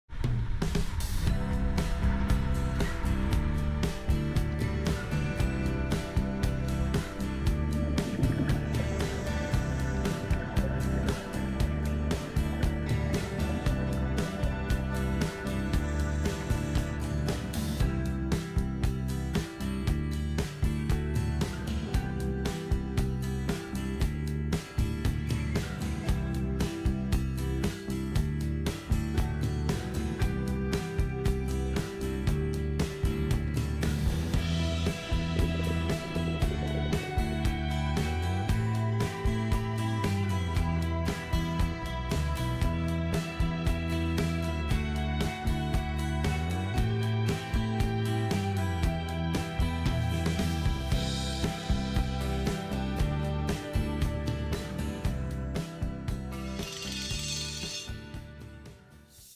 Underwater Travel in Aqaba Jordan, Scuba Diving with Seabike!
Dive trip to Aqaba Jordan (The Red Sea) with Seabike.
The Red Sea is a world class scuba destination with great visibility, vivid corals, millions of fish and entire ship wrecks that have been enveloped with sea life. People come from all over the world to Aqaba, Jordan to join Diva Aqaba on these epic adventures.